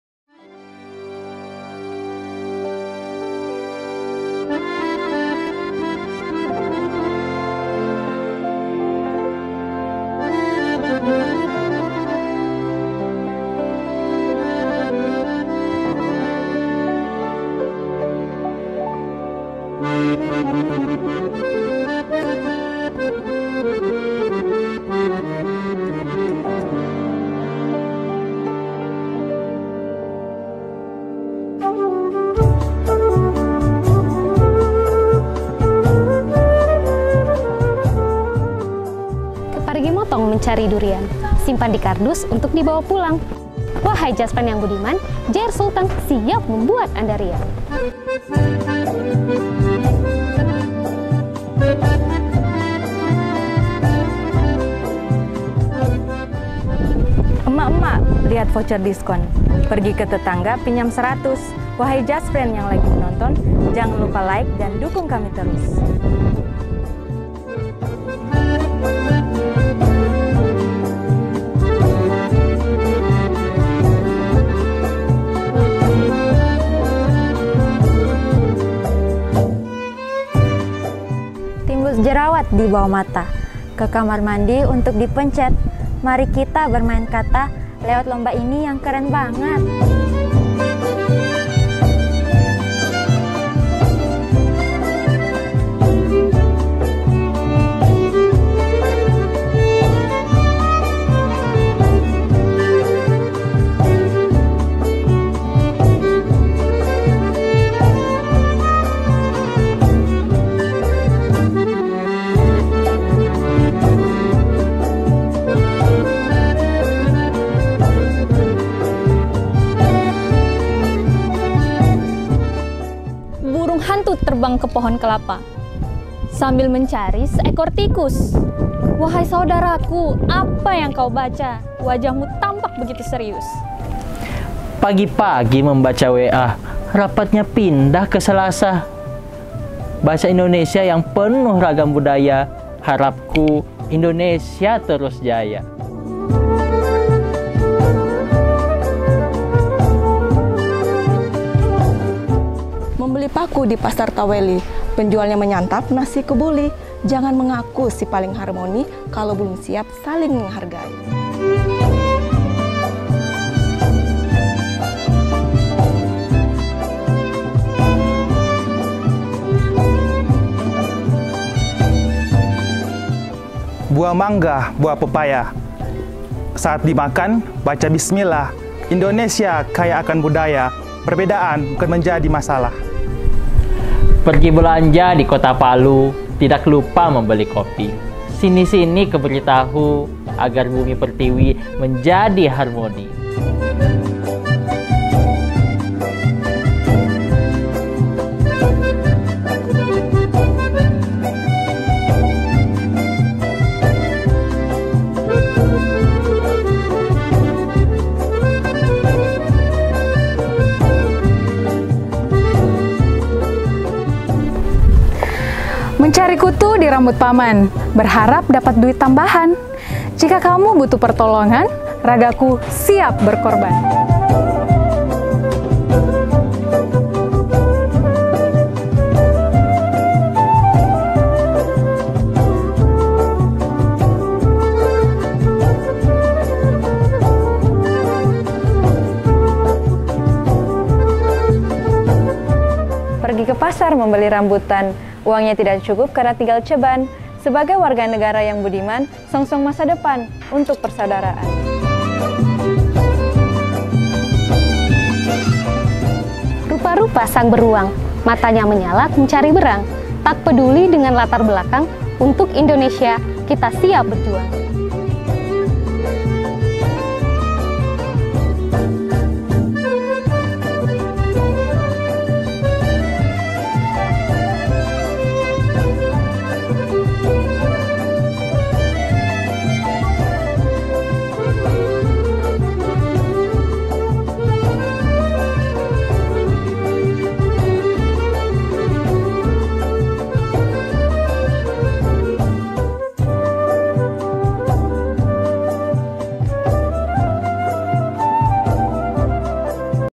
LOMBA BERBALAS PANTUN - TADULAKO RANGERS - CABANG SULAWESI TENGAH
Hai Jasfren dan Somi..
Dalam rangka menyambut HUT JR ke-63, PT Jasa Raharja menyelenggarakan Lomba Berbalas Pantun  bertema "Harmoni untuk Negeri" dengan total hadiah puluhan juta rupiah.

Yuk dukung karya-karya terbaik Insan Jasa Raharja dengan like dan comment ya..!!